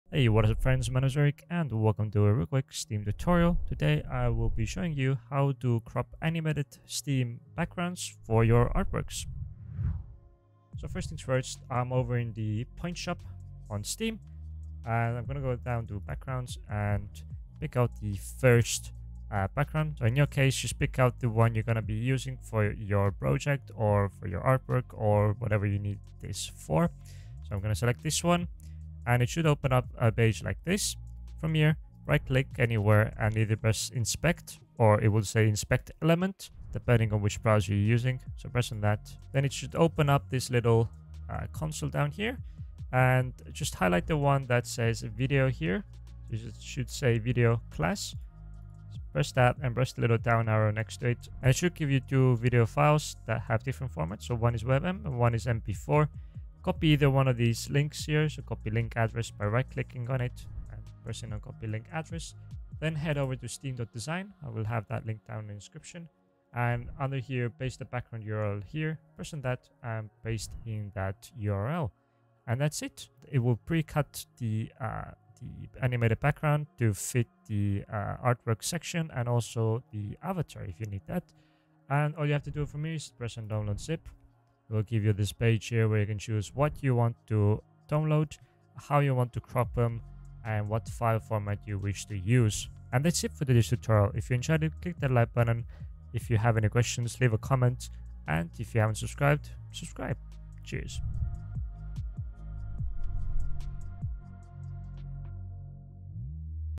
CROP ANIMATED STEAM BACKGROUNDS FOR ARTWORKS! - Steam Tutorial
In this tutorial I show you how to easily crop animated Steam backgrounds, so you can upload them into the Artwork showcase section on your Steam profile!

🎬Timestamps:
Intro: (0:00)
Getting The Background: (0:11)
Getting The Video File: (0:32)
Cropping & Getting the GIF Files: (1:03)
Outro: (1:33)